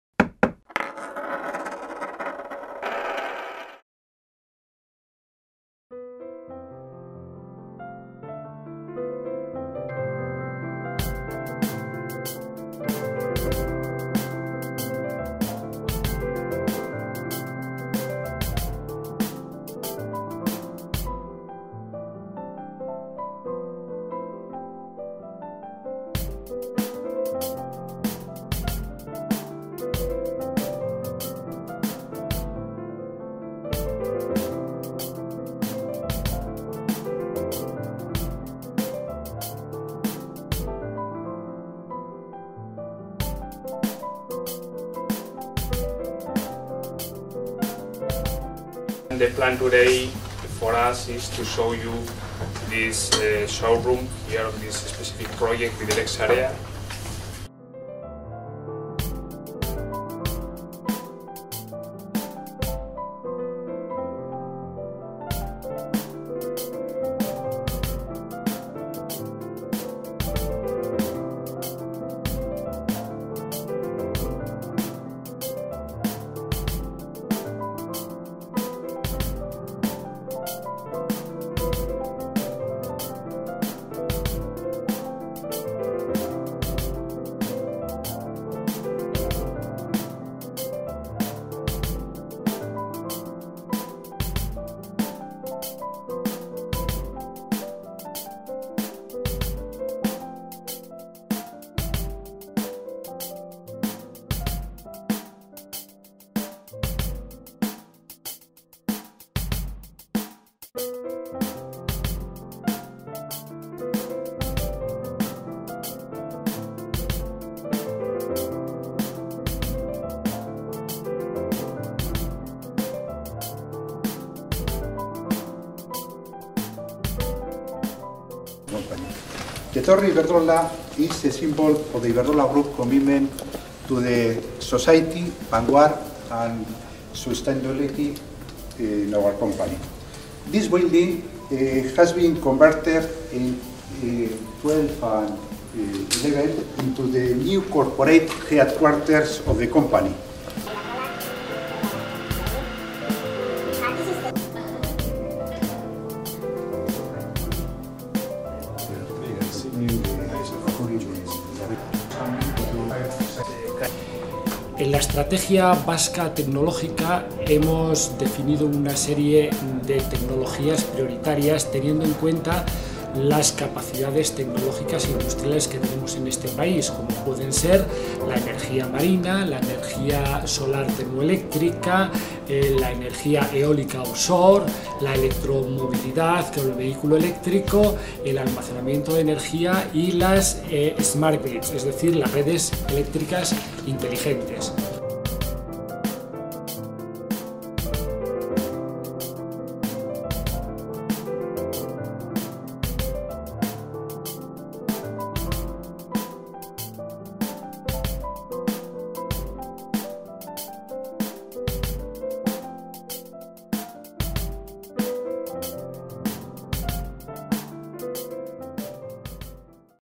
Future Energies Tour - Cleantech Forum Europe 2013
Bilbao ha acogido la novena edición del "Cleantech Forum Europe" del 16 al 18 de abril de 2013.El Cleantech Fórum esta orientado a generar oportunidades de negocio e inversión entre los distintos agentes internacionales y promover el contacto con las principales regiones europeas líderes en innovación tecnológica.